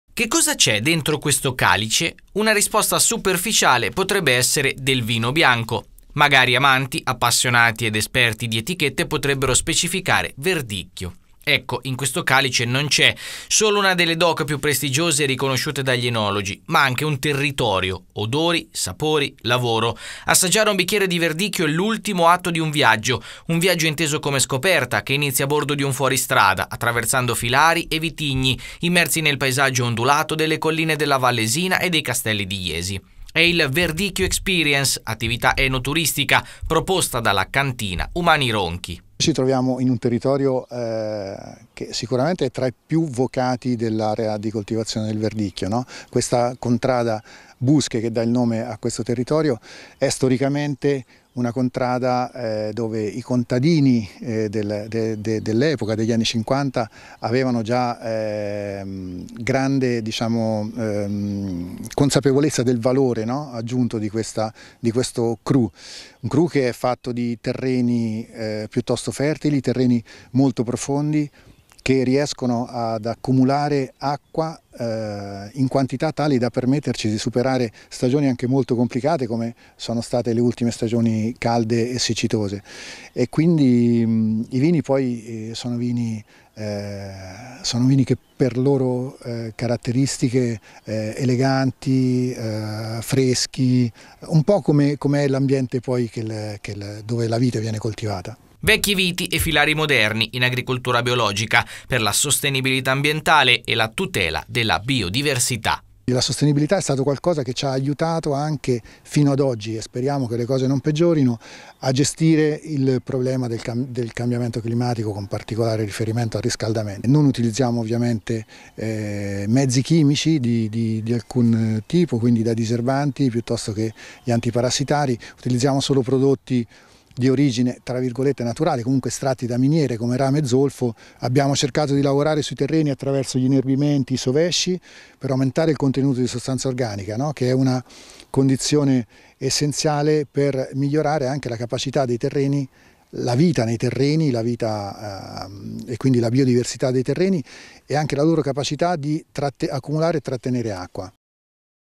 E'TV Umani Ronchi WINE TOUR VERDICCHIO
Il wine tour di Umani Ronchi per scoprire i
segreti del Verdicchio a bordo di un fuoristrada